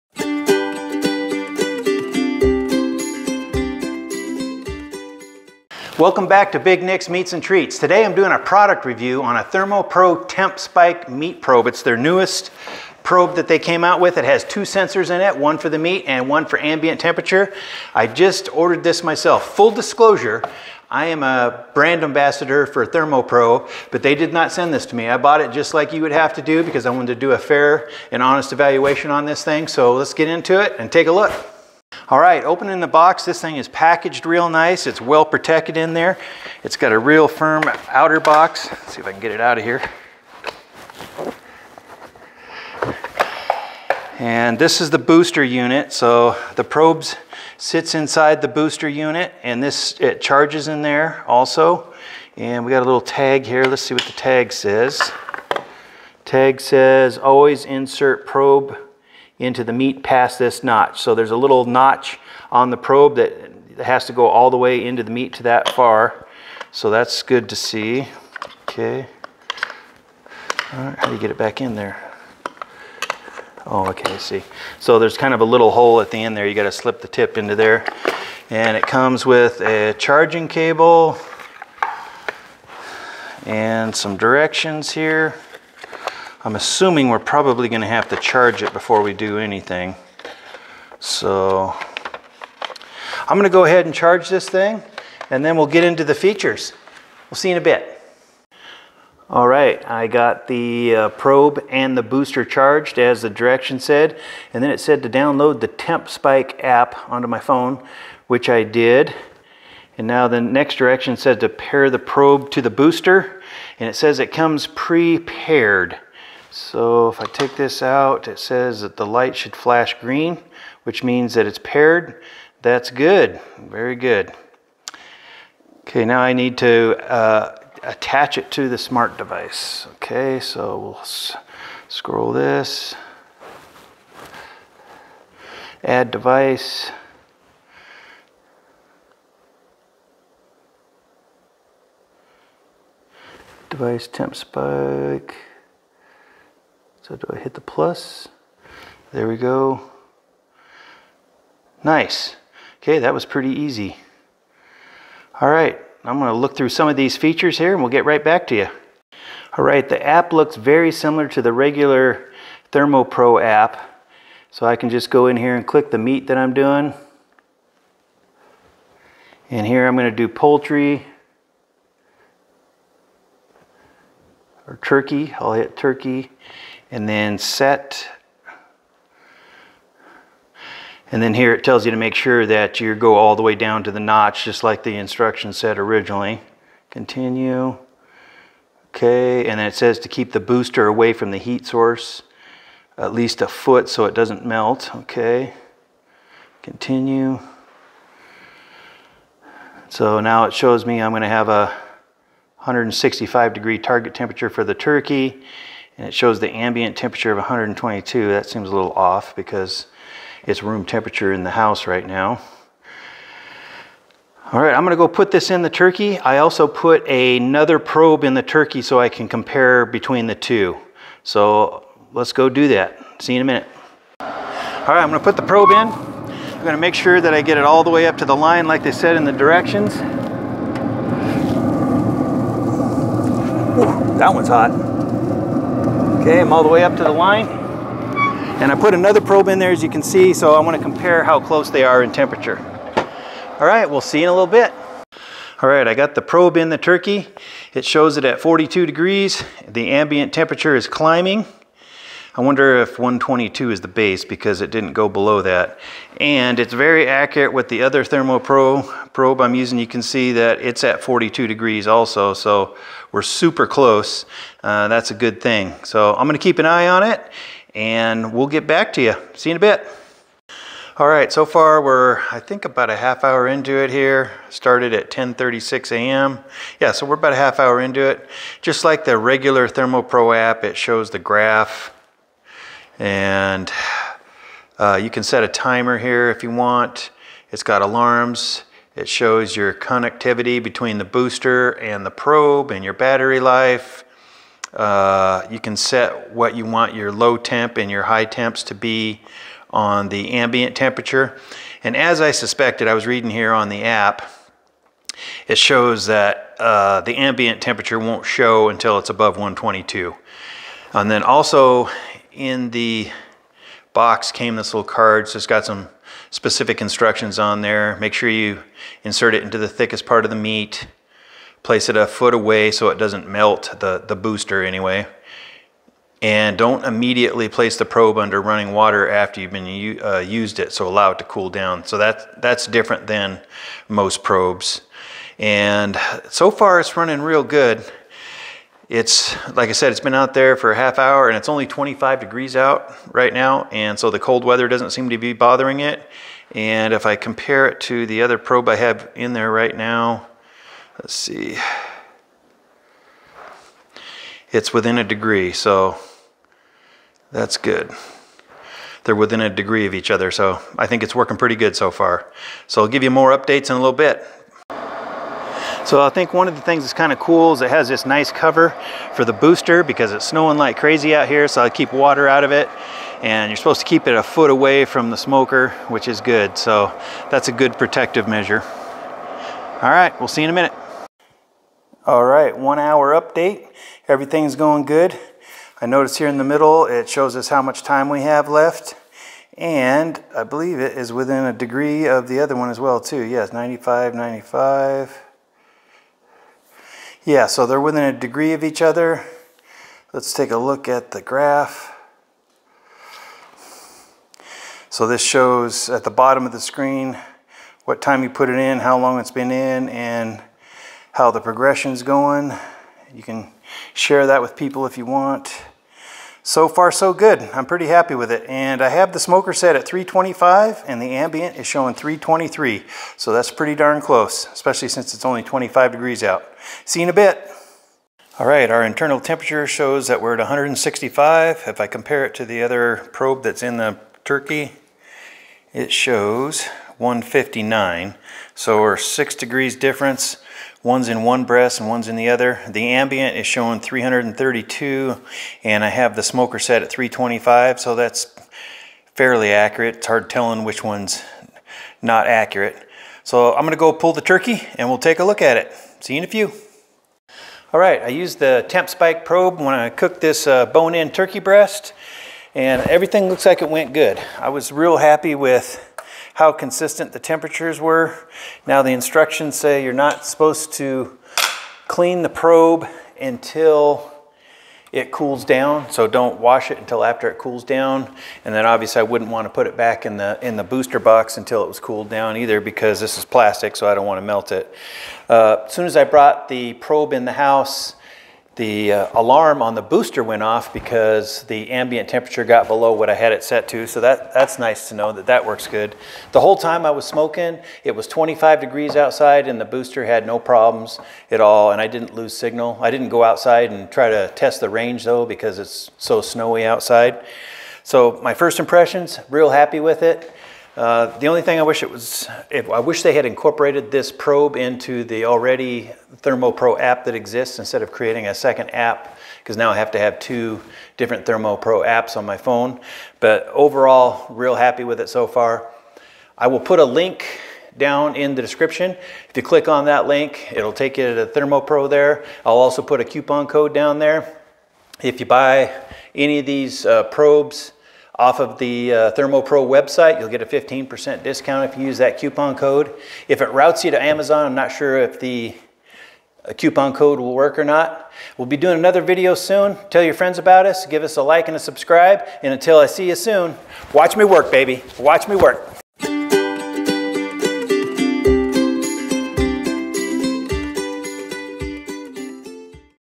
ThermoPro TempSpike Wireless Probe - Unboxing and Testing
I unbox a new ThermoPro TempSpike and test it against a ThermoPro TP25 for accuracy and function.